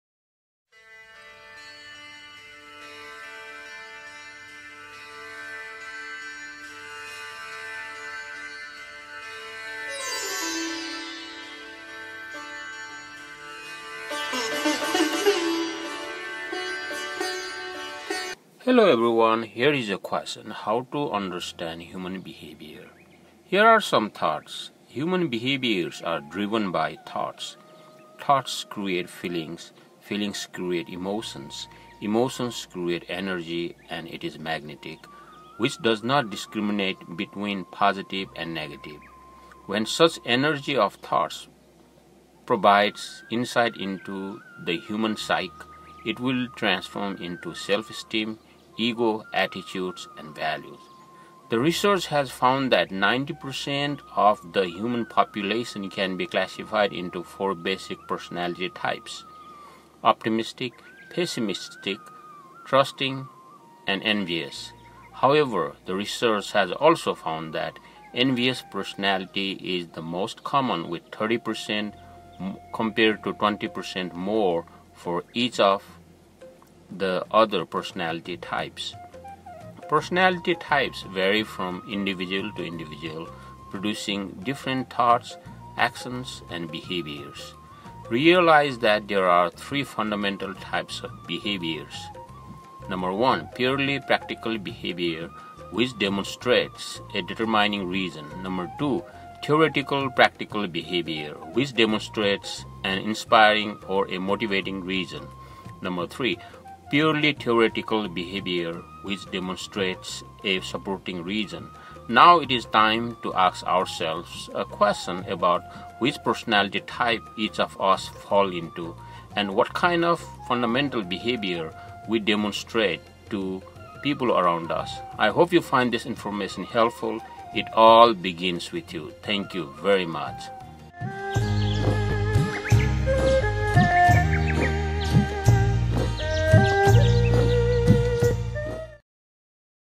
How to understand human behavior? | Episode 152 | Feb 2021
Copyright content of Naturally Engaging Motivator Channel.

Here are some thoughts. Human behaviors are driven by thoughts. Thoughts create feelings. Feelings create emotions. Emotions create energy and it is magnetic which does not discriminate between positive and negative. When such energy of thoughts provides insight into the human psyche, it will transform into self-esteem, ego, attitudes, and values.

The research has found that 90% of the human population can be classified into four basic personality types: Optimistic, pessimistic, trusting, and envious. However, the research has also found that envious personality is the most common with 30% compared to 20% more for each of the other personality types. Personality types vary from individual to individual, producing different thoughts, actions, and behavior.

Realize that there are three fundamental types of behaviors. 1)  Purely practical behavior which demonstrates a determining reason. 2) theoretical-practical behavior which demonstrates an inspiring or a motivating reason 3) Purely theoretical behavior demonstrates a supporting reason.

Now it is time to ask ourselves a question about which personality type each of us falls into and what kind of fundamental behavior we demonstrate to people around us. I hope you find this information helpful. It all begins with you.